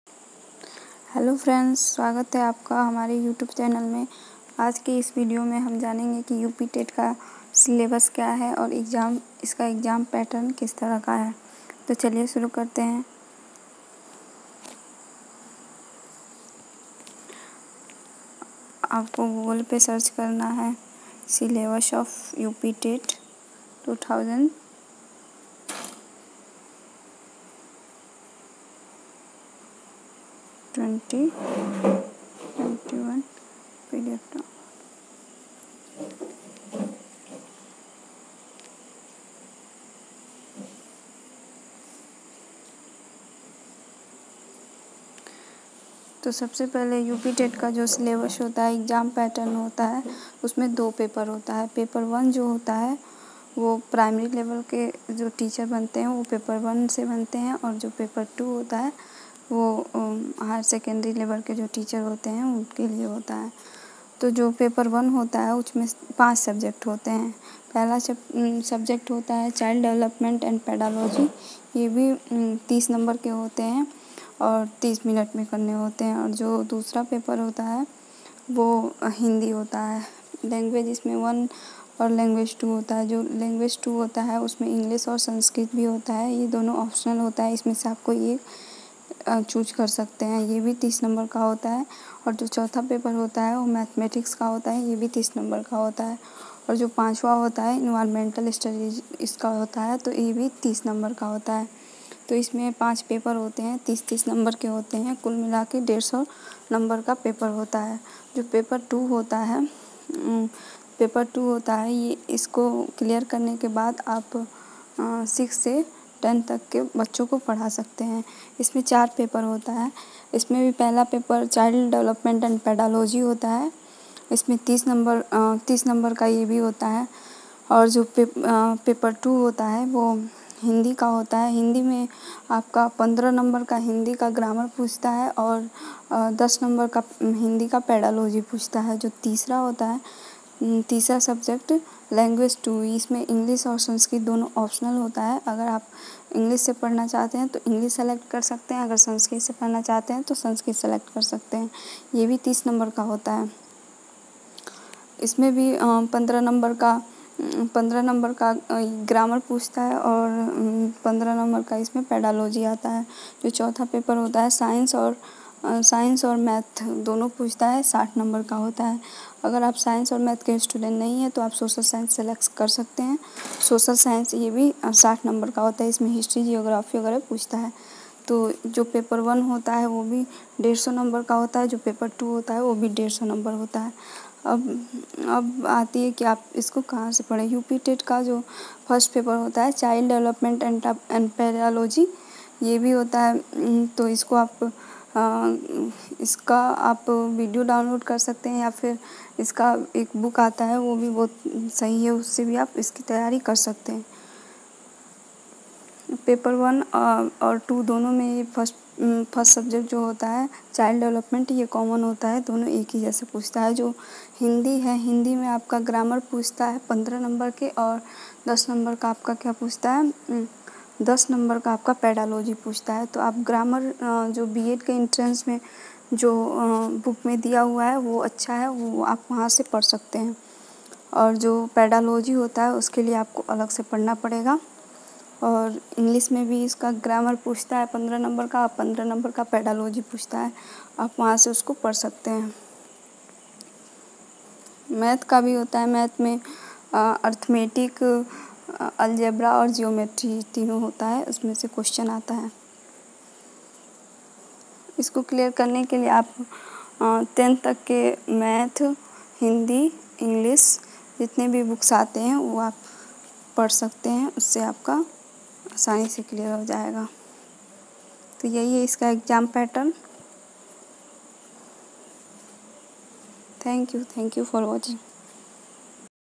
Syllabus of UPTET 2021...
Hello friends...aaj k is video m mai aapko uptet k exam pattern aur syllabus k bare m btaungi